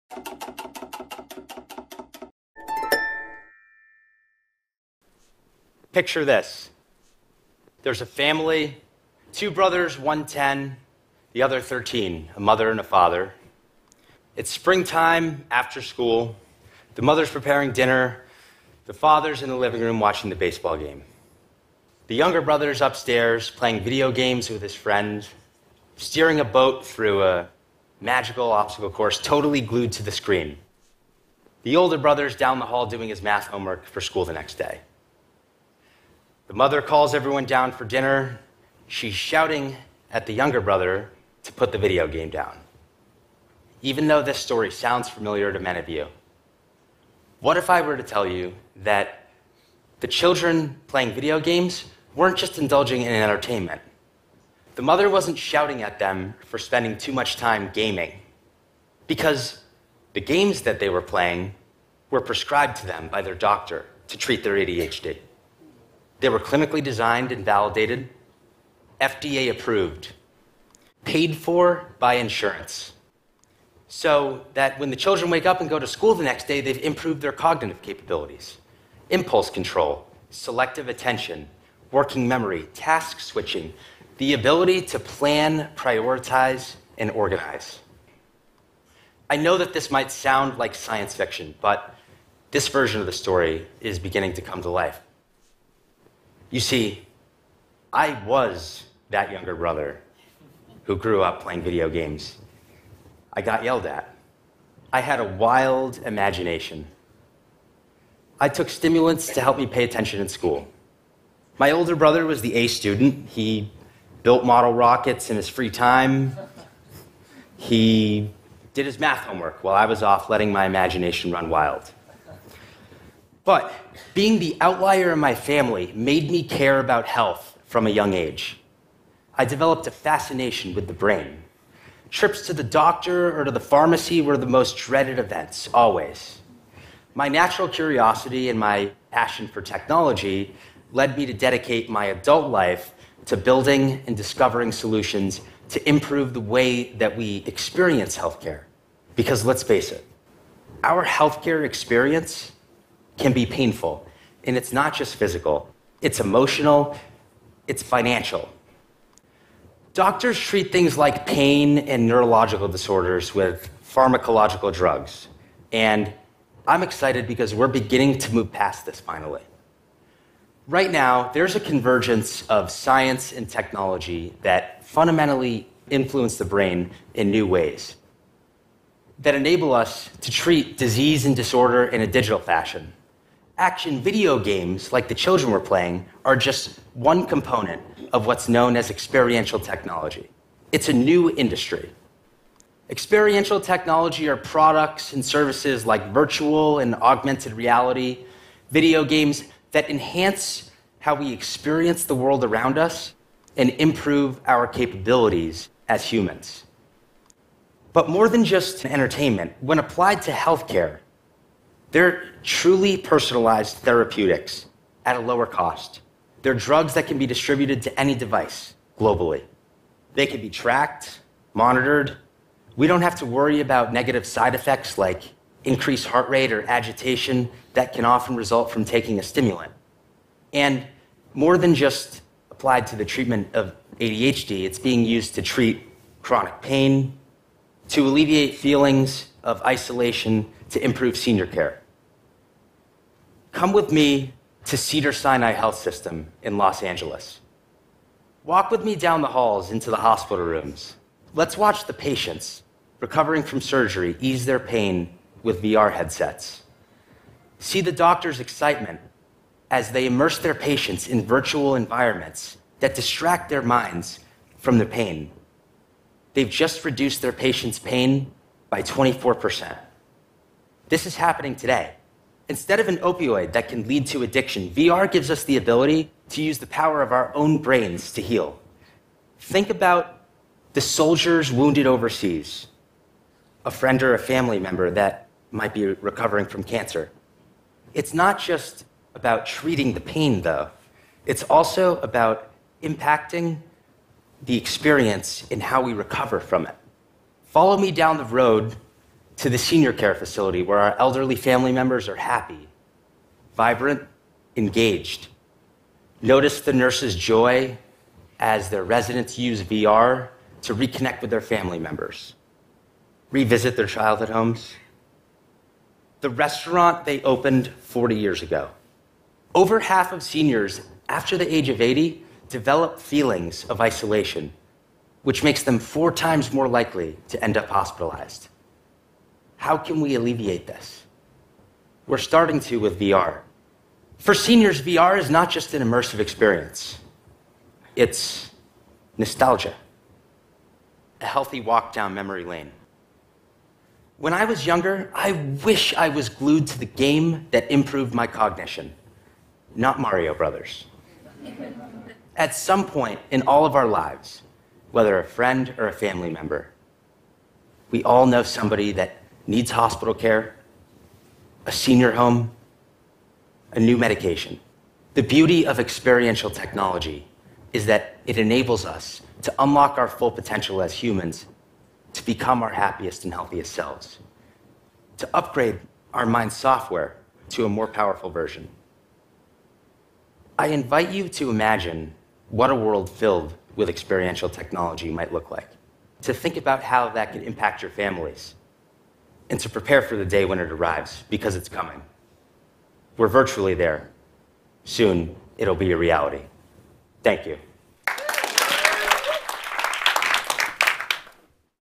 Jordan Brown: Imagining a future for digital medicine
Imagine a world where doctors prescribe video games as medicine, or patients alleviate pain with VR. A convergence of neuroscience and technology is ushering in a new era of digital medicine. Experiential technology products and services like Immersive Video Games and Virtual Reality, are more than entertainment products: they enable us to improve our physical and mental capabilities, and enhance the way we experience the world around us. Jordan Brown, a digital health entrepreneur, and innovation consultant, explores what a world filled with experiential technology can do to improve patient care, and how it can impact the health and wellbeing of our families.